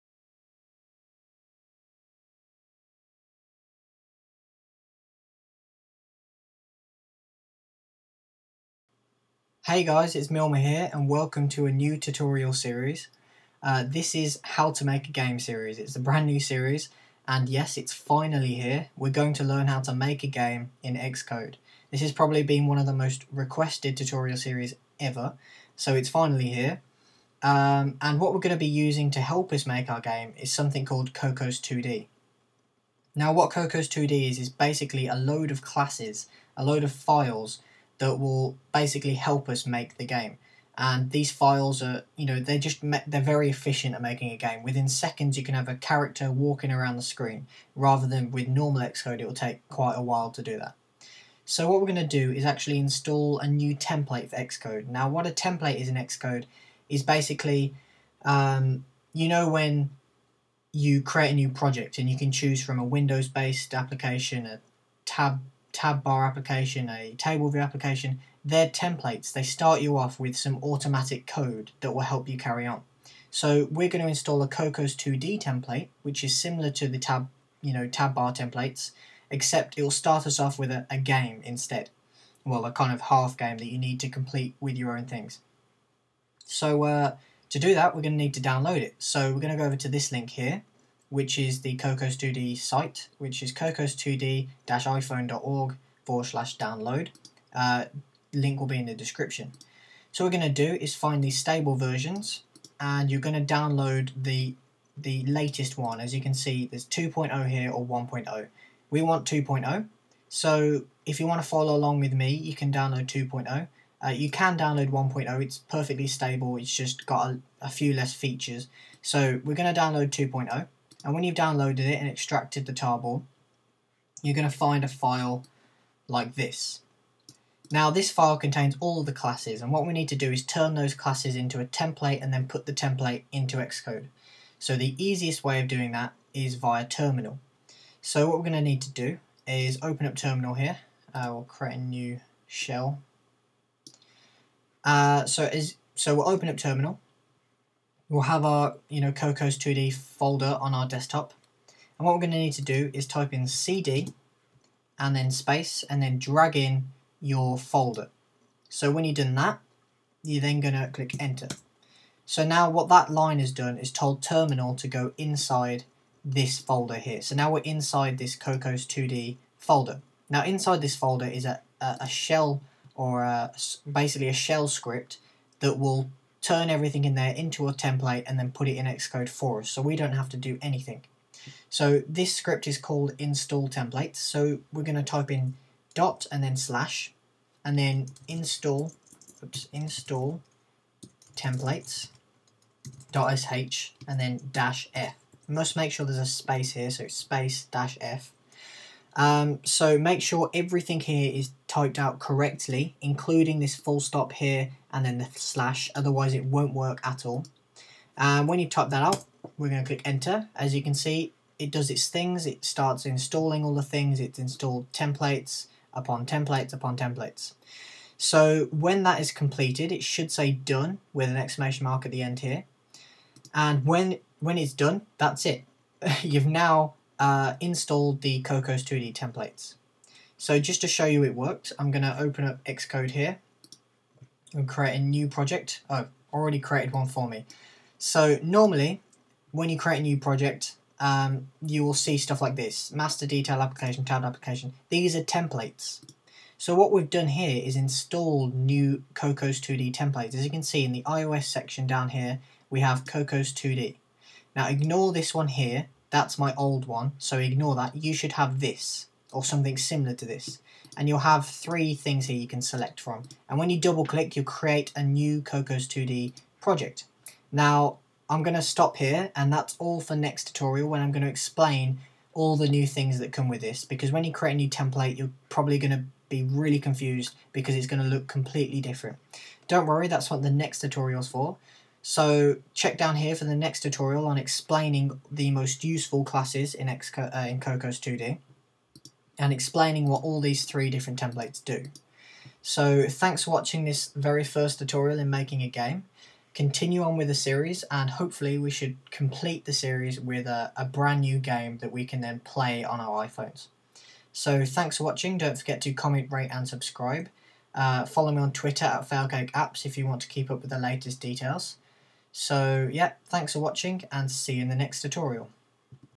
iPhone Programming: Making a Game - Installing Cocos2D
It's finally here. How to make a game for iOS! In this tutorial I teach you how to install one of our most important tools, Cocos2D.

Terminal Code:
    cd /Desktop/cocos2d-iphone-2.0/
    ./install-templates.sh -f